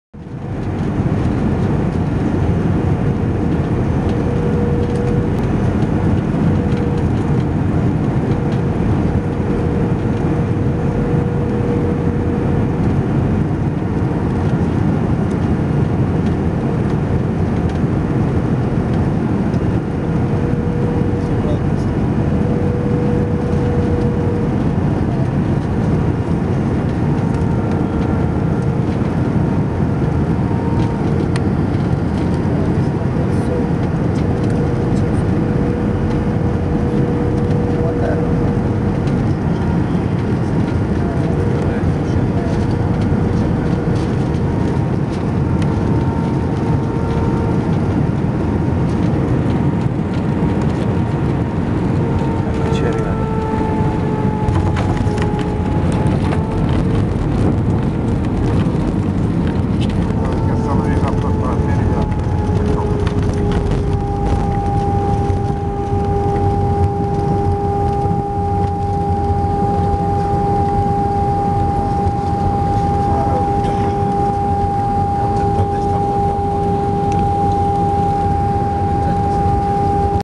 atterraggio a palermo
volo AZ1775 da linate con Airbus A321